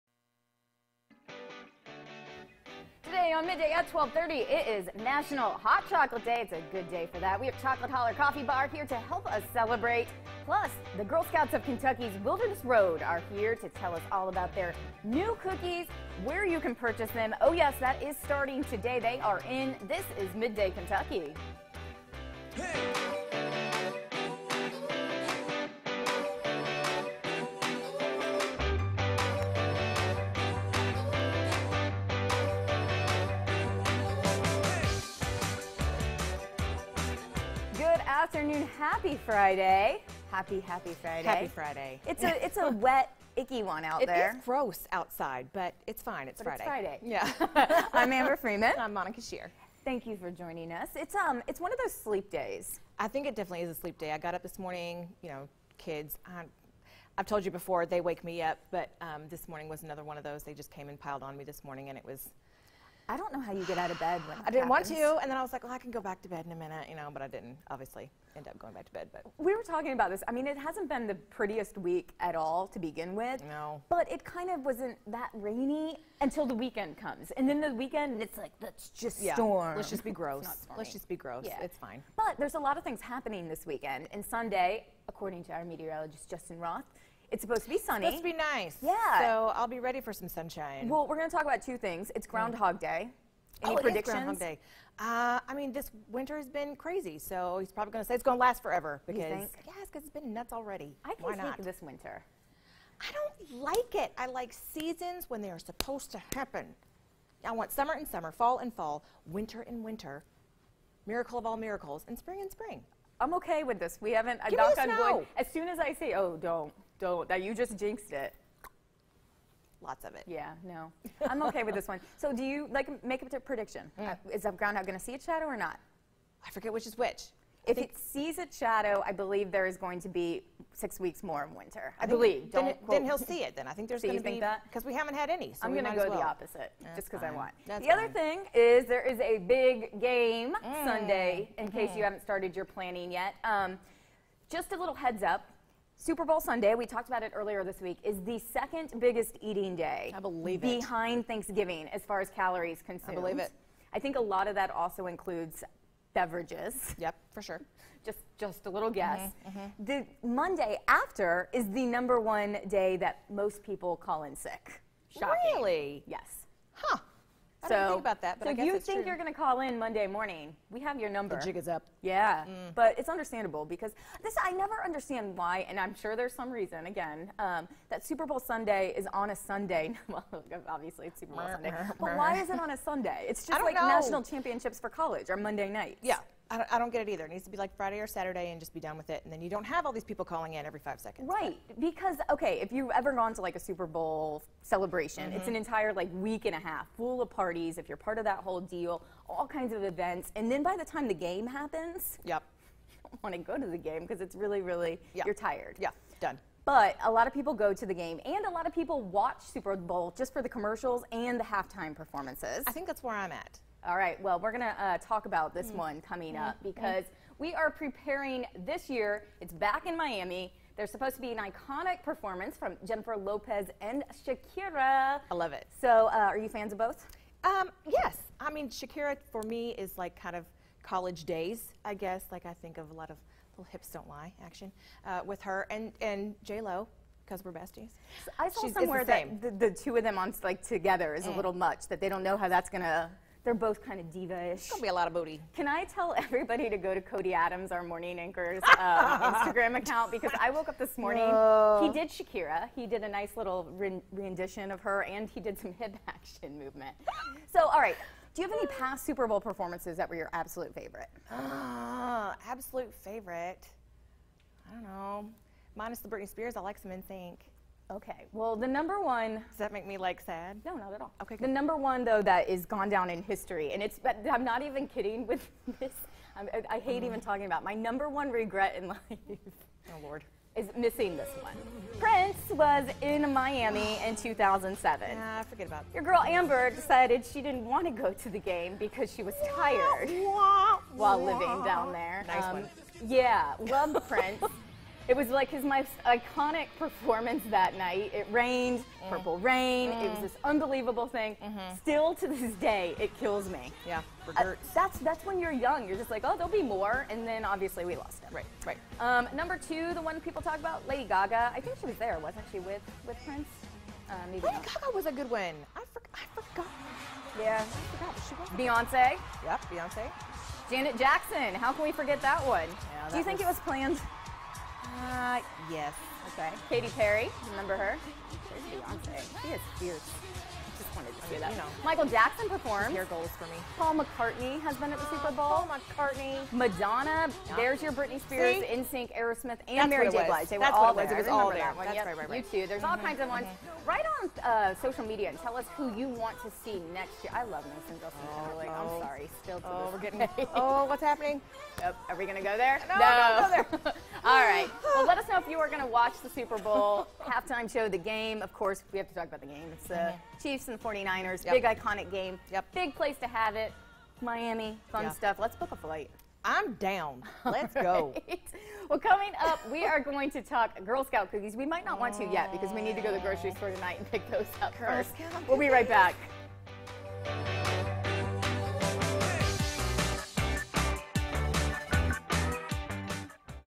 Table Talk - Super Bowl Performances
The big game is Sunday! Let's face it, many of us watch to see the amazing half-time performances. Join us as we reflect on some of the best ones!